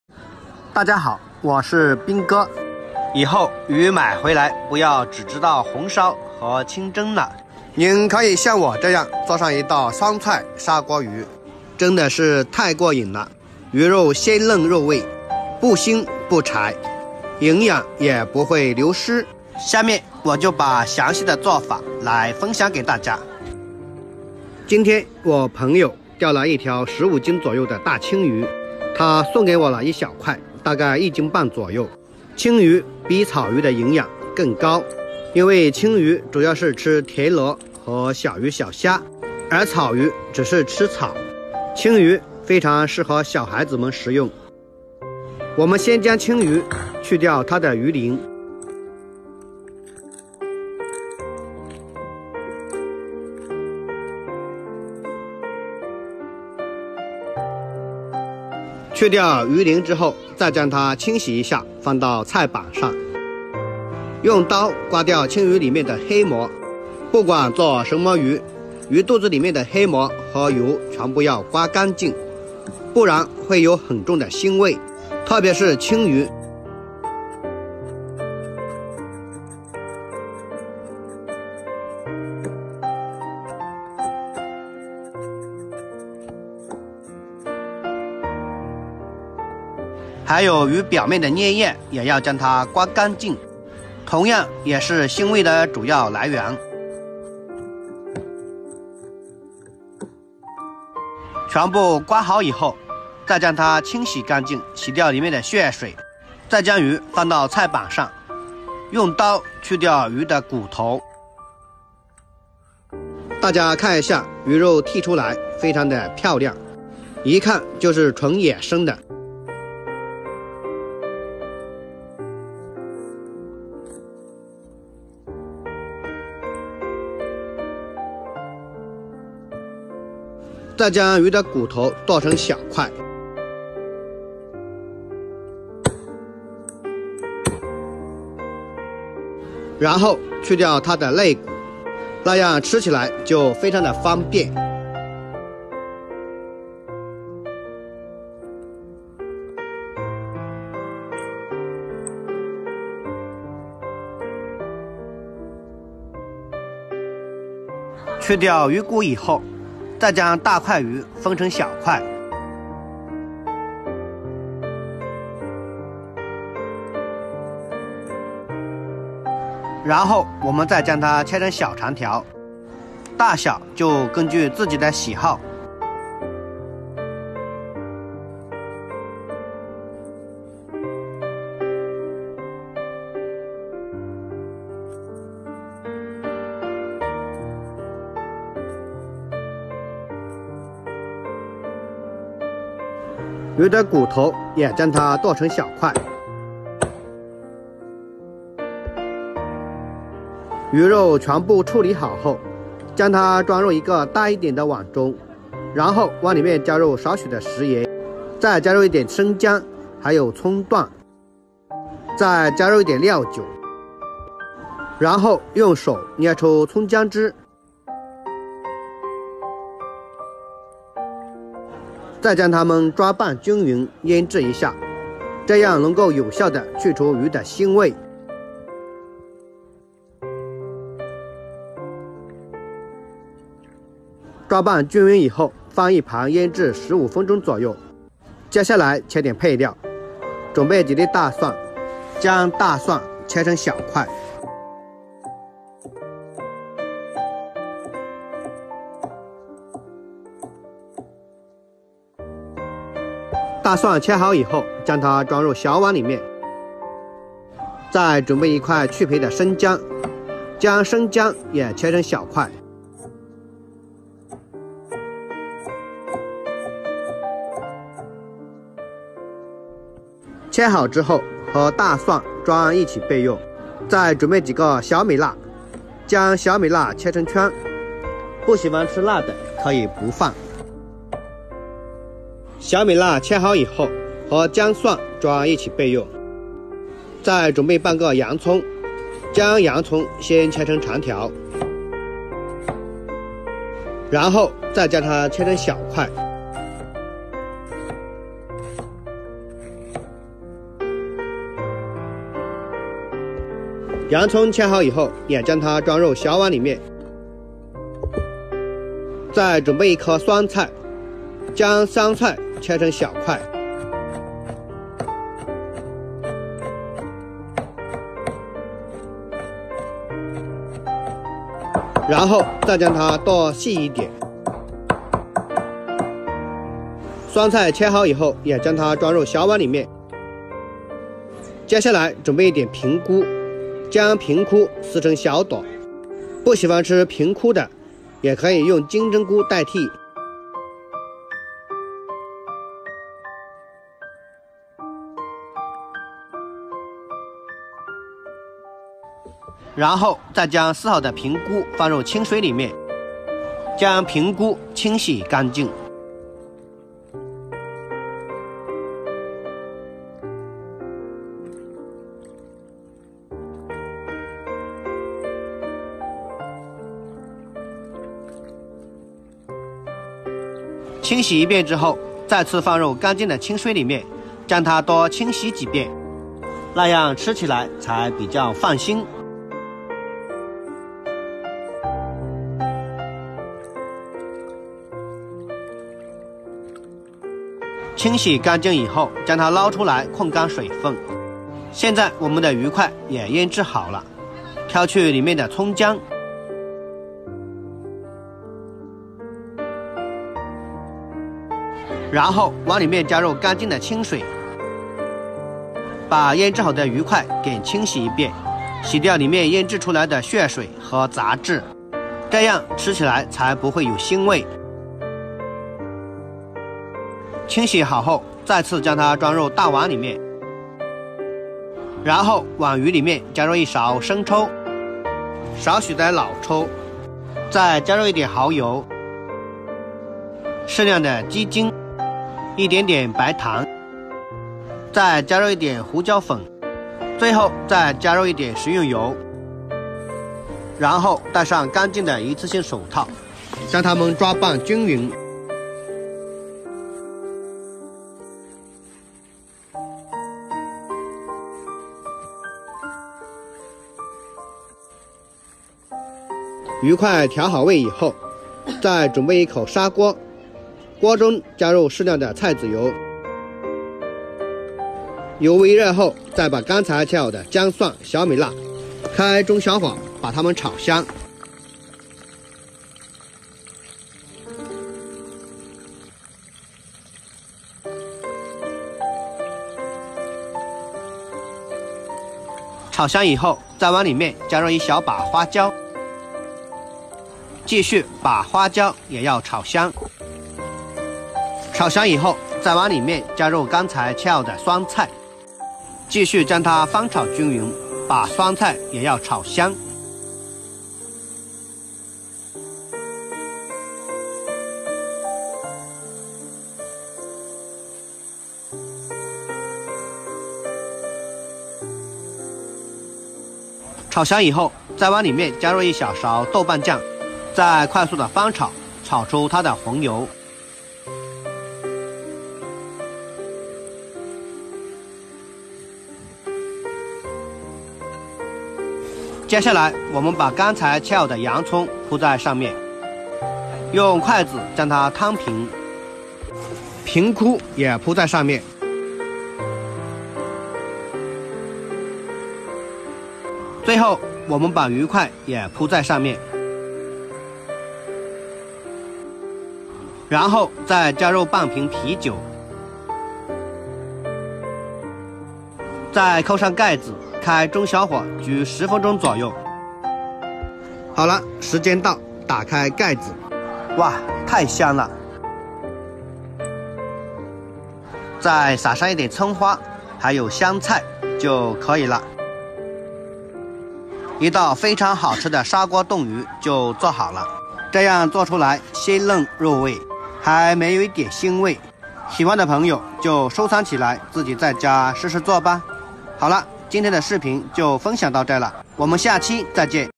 青魚這樣做著的太過癮了，魚肉鮮嫩入味，開胃下飯還沒一點腥味【胡斌哥美食】青魚
大家好，歡迎大家來到胡斌哥美食。

相信熱愛美食的朋友也都是熱愛生活的！

簡易食譜,新手下廚,一日三餐,早餐午餐晚餐,